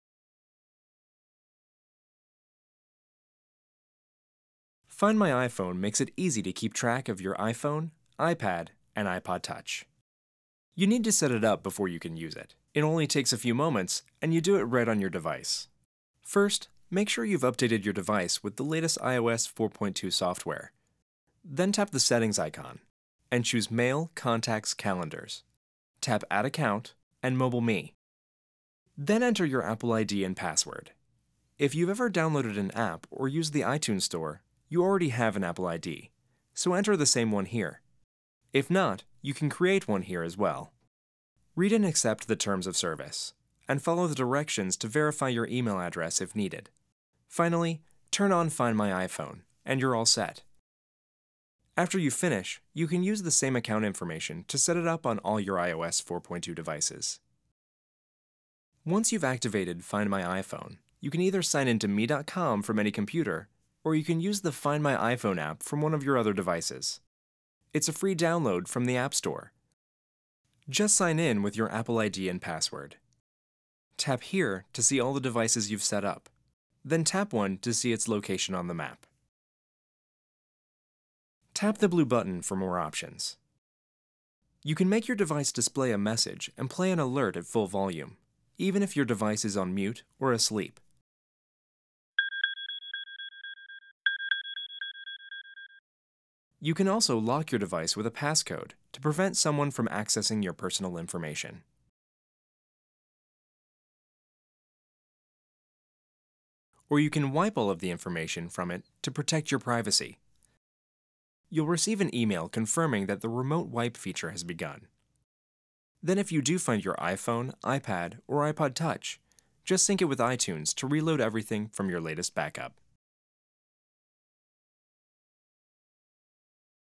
How To Set Up Find My iPhone
1. Make sure your iPhone is running iOS 4.2 or later. If you're creating a free account for the first time, you need an iPhone 4.
2. Tap the Settings icon and choose Mail, Contacts, Calendars.
3. Tap the Add Account button and select MobileMe.
4. Enter your Apple ID and password.

If you have an iTunes Store account or have ordered from the Apple Online Store, you already have an Apple ID.

If you don't have an Apple ID, tap Create Free Apple ID and follow the onscreen instructions.

5. Verify your account.

If your MobileMe account says "Not Verified," check your email inbox for a message from Apple. Click Verify Now and sign in with your Apple ID.

If you don't see a verification email in your inbox, select your MobileMe account and tap Resend Verification Email.
6. Return to the MobileMe screen and switch on Find My iPhone.

When the Find My iPhone message appears, tap Allow.